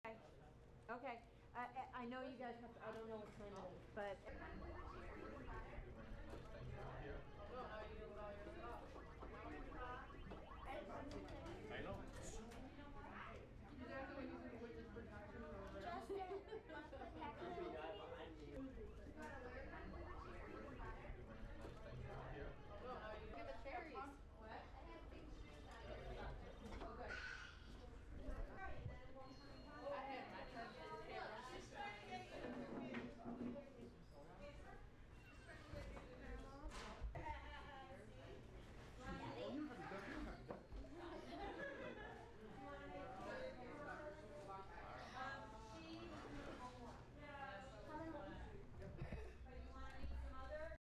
Dream Factory helps fulfill sick teen's wish
The Dream Factory helps makes the dreams of children with critical or chronic illnesses come true. On Sunday, a Spencerport teen had his wish granted at Linda's New York Pizzeria on Lyell Avenue.14-year-old Justin Samis has the blood disorder hemophilia meaning he has to endure regular infusions.